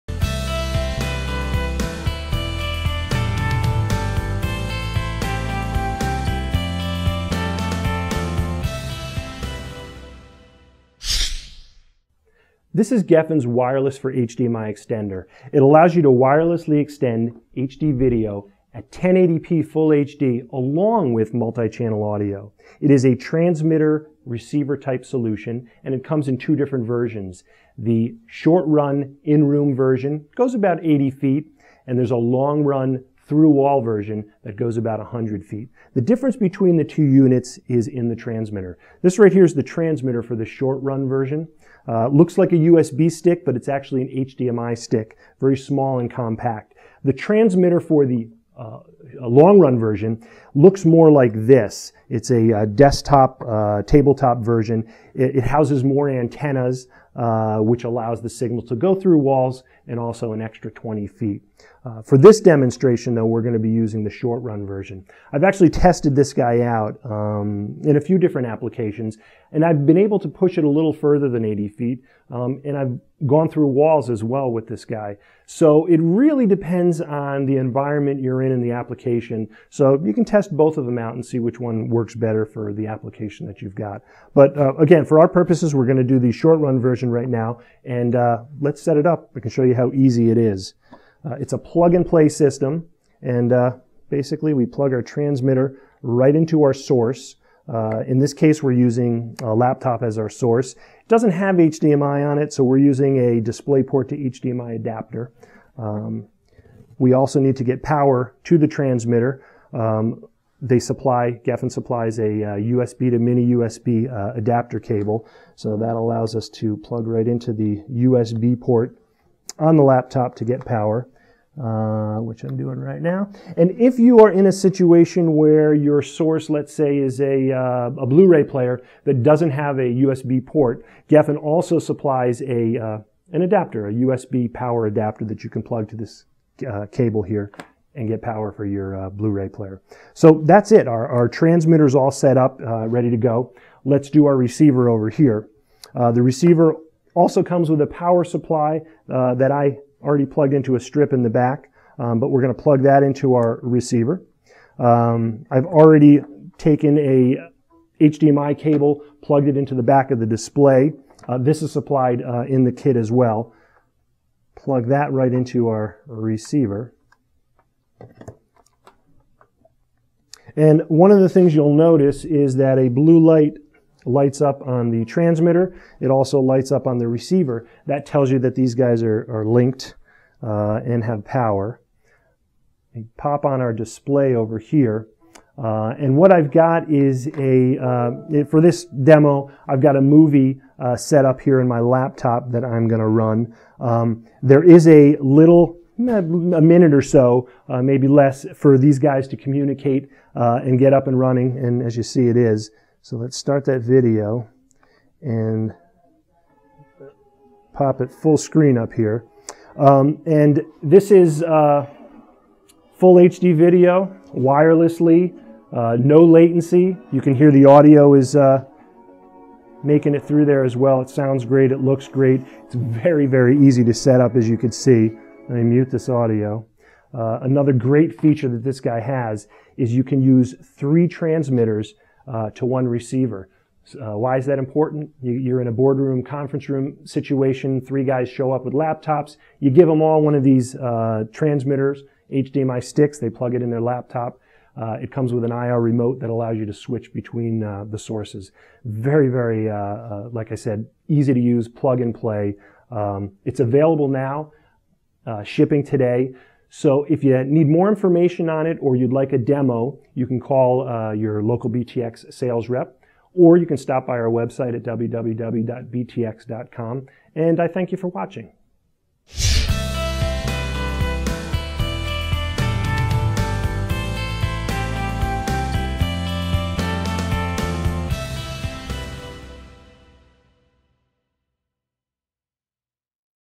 Gefen - Wireless for HDMI
This NEW Wireless technology extends HD video at 1080p Full HD with multichannel digital audio up to 80 or 100 feet. This wireless product is comprised of a Sender Unit and Receiver Unit. Up to three Sender Units can be used for each Receiver Unit. The solution comes with a remote control to allow switching between Sender Units.